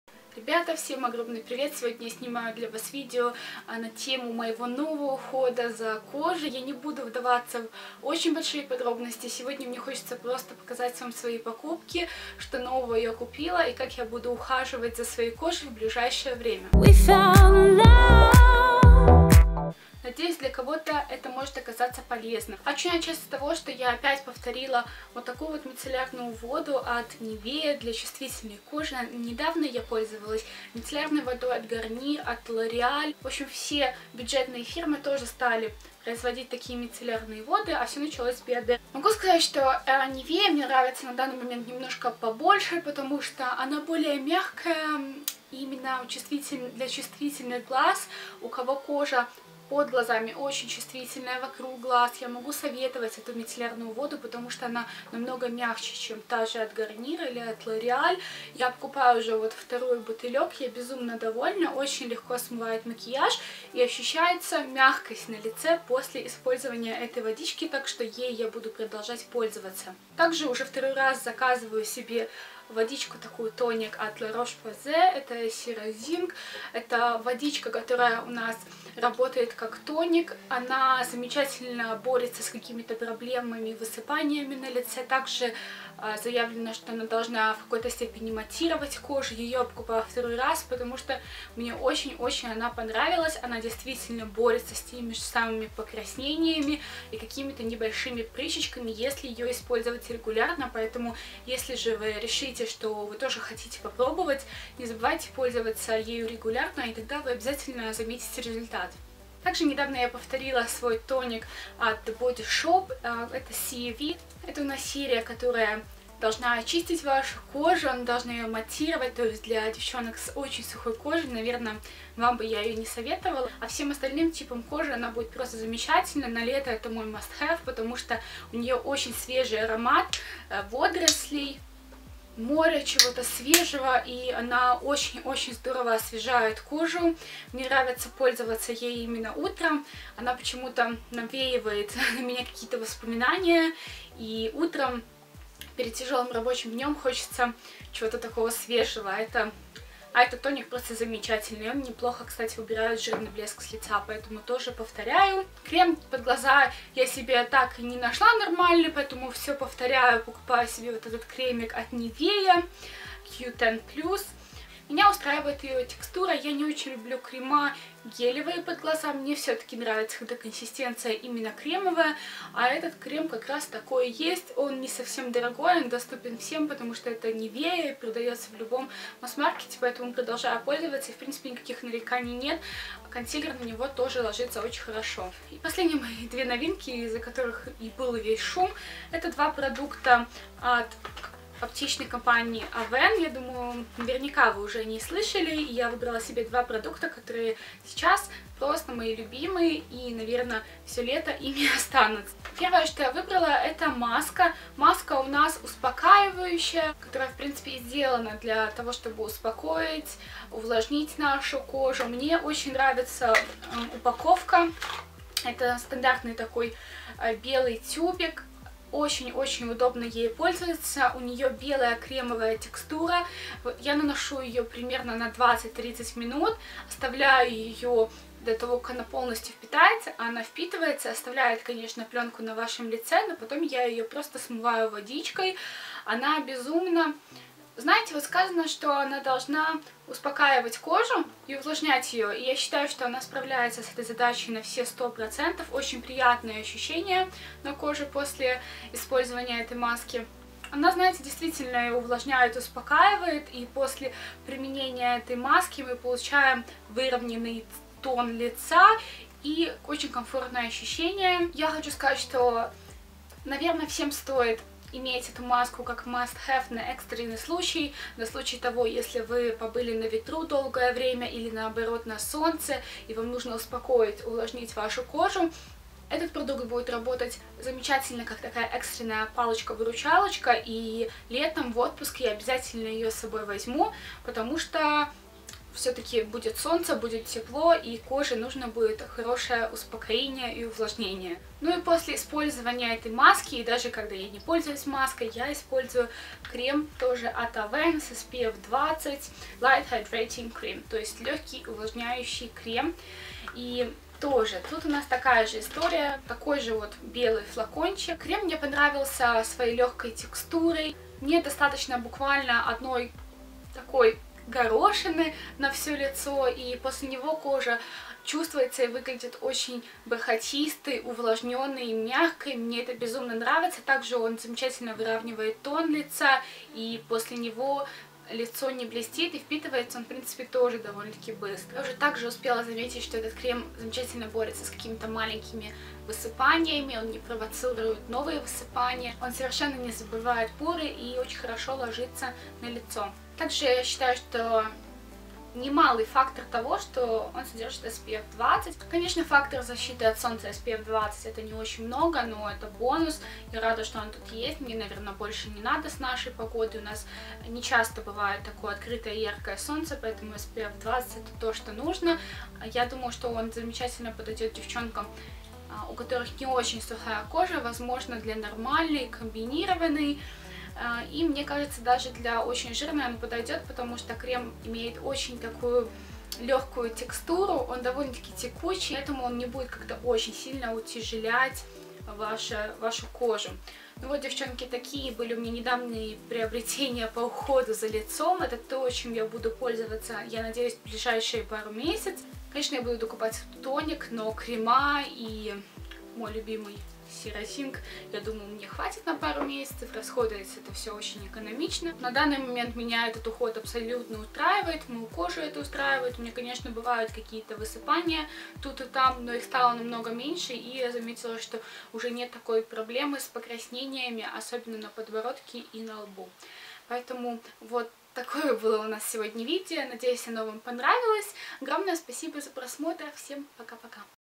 Мой уход за кожей | Покупки Avene, Nivea, La Roche Possay
Привет дорогие мои. Сегодня я показываю вам как я ухаживаю за своей кожей. Я прикупила себе несколько новых продуктов в аптеке и сегодня я хочу вам их показать

1.Nivea Sensitive 3 in 1 Micellar Cleansing Water
2.La Roche-Posay Serozinc Zinc Sulfate Solution
3.NIVEA Q10 Plus Anti-Wrinkle Eye Cream
4.Avene Soothing Moisture Mask
5.Eau Thermale Avène Hydrance Optimale UV Light SPF20